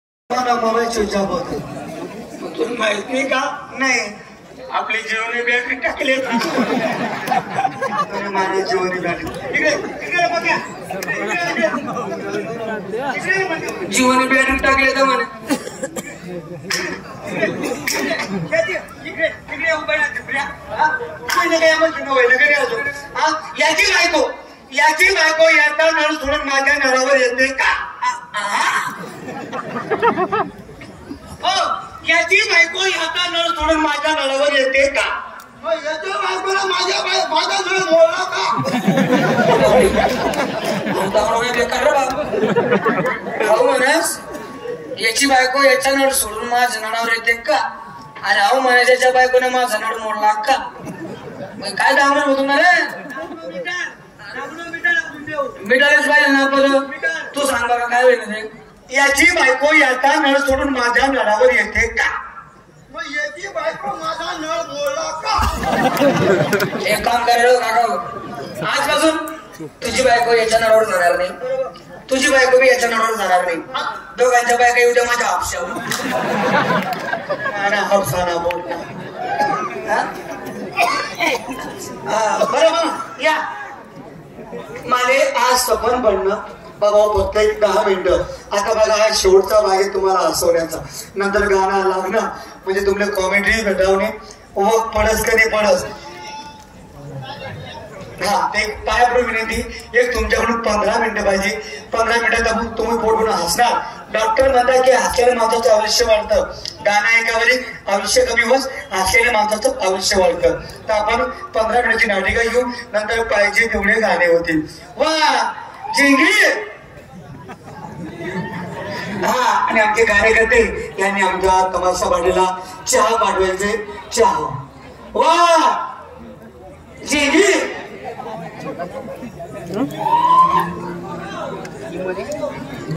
प्यारेलाल वाघ आणि उमेश कोतकर ची कॉमेडी || नगरदेवळेकर लोकनाट्य तमाशा मंडळ
उमेश कोतकर आणि प्यारेलाल वाघ, सुरज, प्रकाश, यांची खूप जबरदस्त कॉमेडी 😁😂🤣

ajay s creation 😊